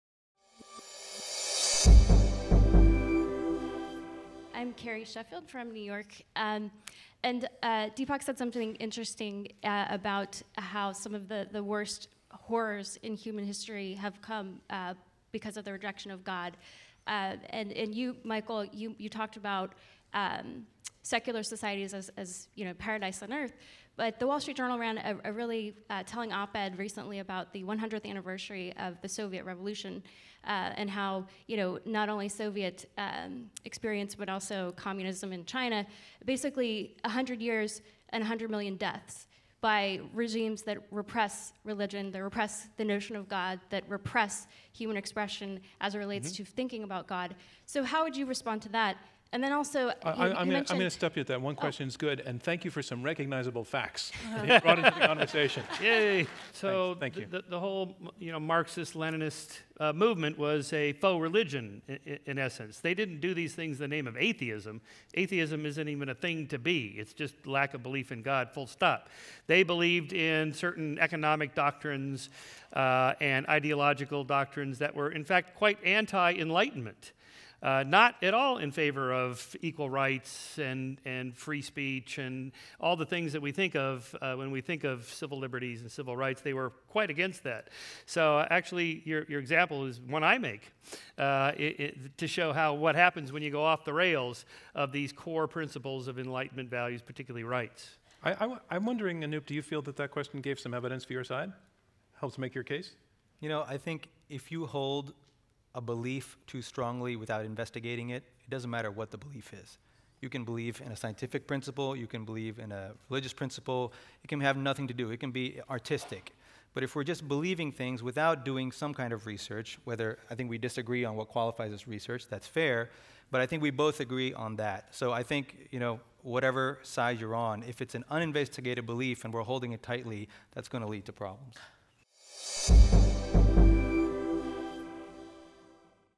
Peace & Religion || Debate Clip || The More We Evolve, The Less We Need God
Does repressing belief in God lead to violence? Heather Berlin, Michael Shermer, and Anoop Kumar respond. An excerpt from the debate "The More We Evolve, the Less We Need God."